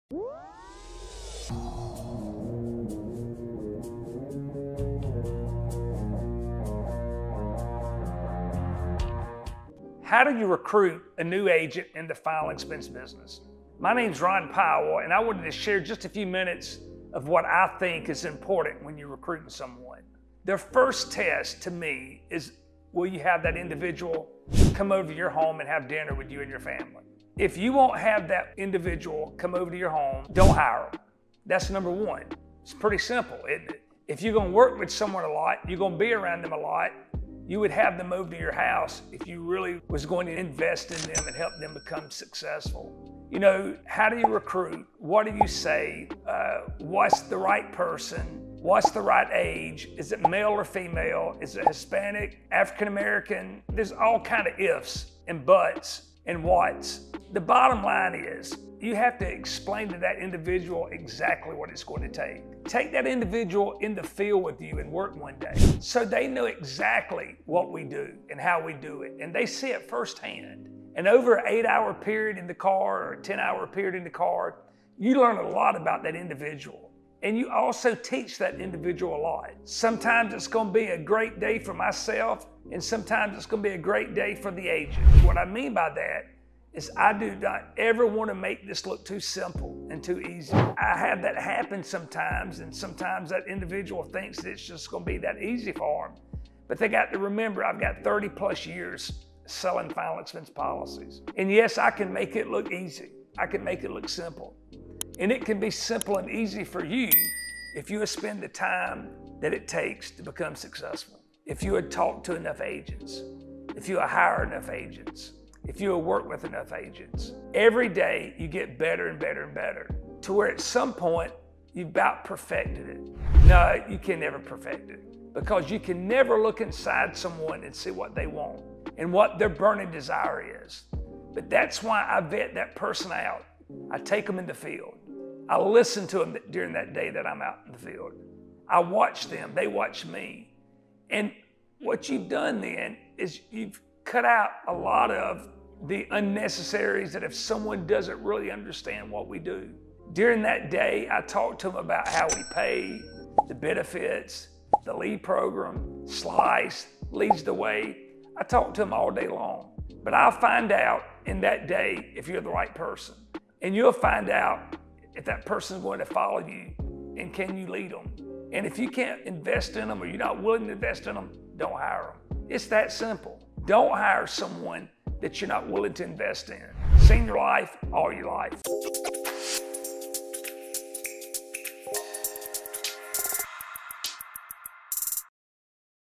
Ron Powell's Winning Formula: How to Recruit The Right Agents
Join Ron Powell, as he unveils his key strategies for recruiting the right agents. In this video, Ron emphasizes the importance of personal connection, advocating for hiring individuals you'd comfortably invite to your home. He shares insights on assessing potential recruits through real-world experiences, highlighting the necessity of transparency and hands-on learning in the field. With over 30 years of experience, Ron provides valuable tips on identifying candidates who are not just willing to join, but are also eager to learn and grow. Discover how to build a successful team by investing in people who align with your values and vision in the final expense insurance business.
Contact Senior Life Today!

Follow Senior Life Insurance Company on Social.